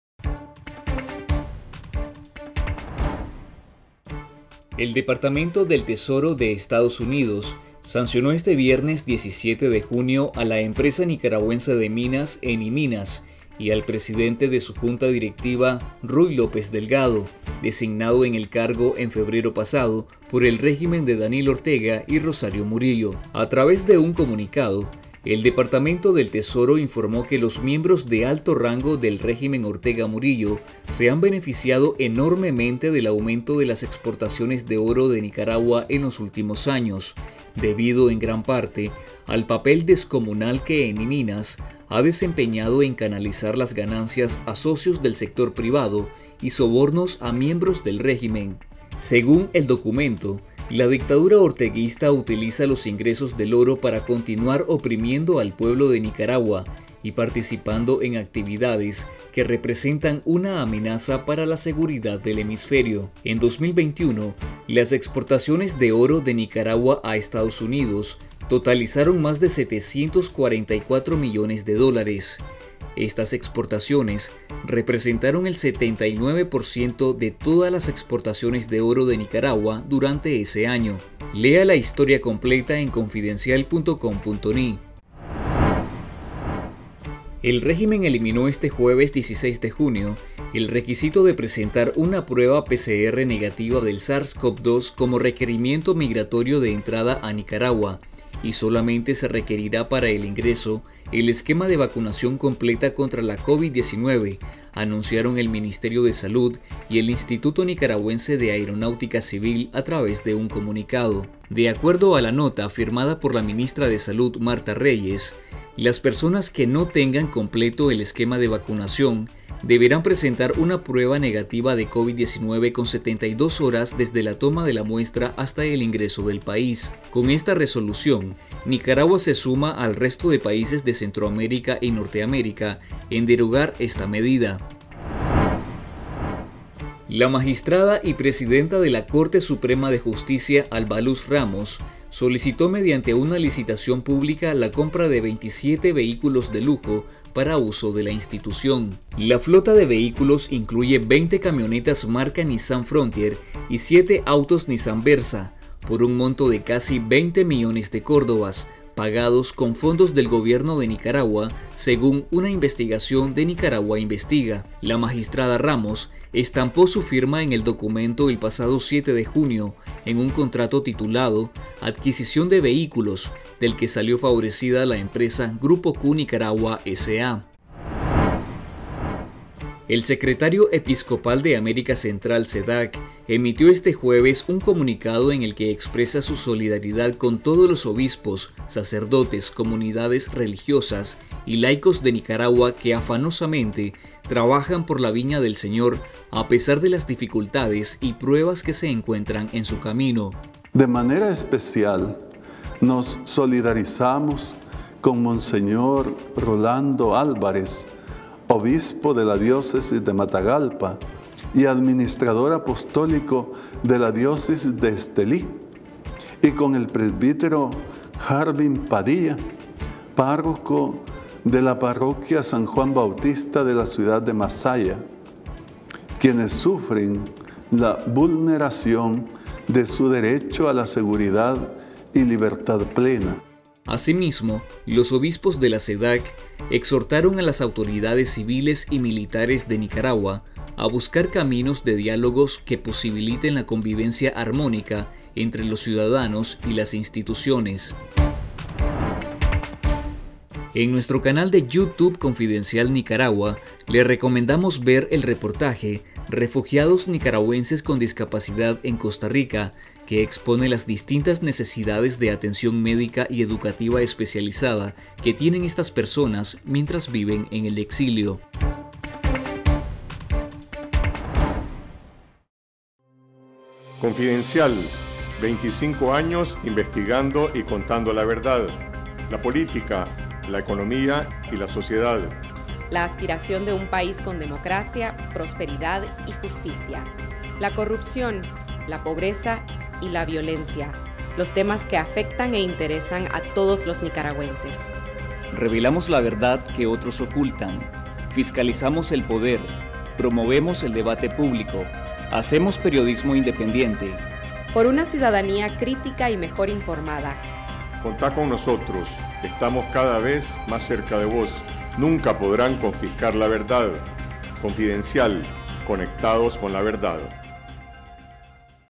EE.UU. sanciona a la Empresa Nicaragüense de Minas y a su presidente ejecutivo
El Departamento del Tesoro de Estados Unidos sancionó este viernes 17 de junio a la Empresa Nicaragüense de Minas, Eniminas, y al presidente de su Junta Directiva, Ruy López Delgado, designado en el cargo en febrero pasado por el régimen de Daniel Ortega y Rosario Murillo.
A través de un comunicado, el Departamento de Tesoro informó que “los miembros de alto rango del régimen Ortega-Murillo se han beneficiado enormemente del aumento de las exportaciones de oro de Nicaragua en los últimos años, debido en gran parte al papel descomunal que Eniminas ha desempeñado en canalizar las ganancias a socios del sector privado y sobornos a miembros del régimen”.

Según el documento, la dictadura orteguista utiliza “los ingresos del oro para continuar oprimiendo al pueblo de Nicaragua y participando en actividades que representan una amenaza para la seguridad del hemisferio”.